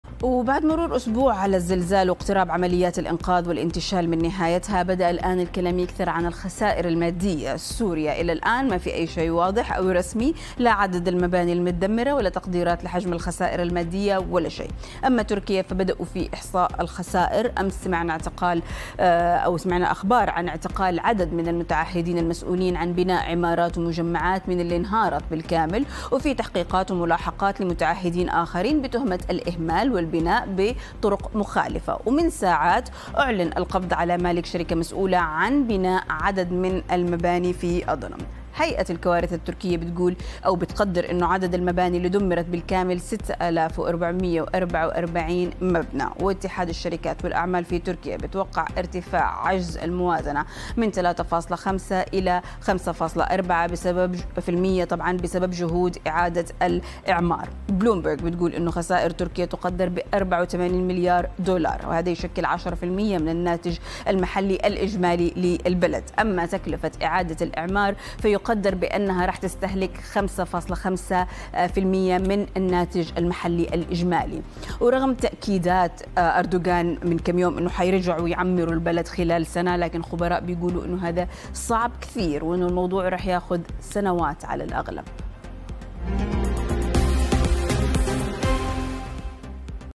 تفاعلكم : تركيا تبدأ بإحصاء الخسائر المادية والقبض على المسؤولين
الحكومة التركية تبدأ بملاحقة المسؤولين عن الزلزال.. وهيئة الكوارث التركية: 6444 مبنى دمر بالكامل جراء الزلزال! وبلومبرغ تقدر خسائر تركيا بـ 84 مليار دولار.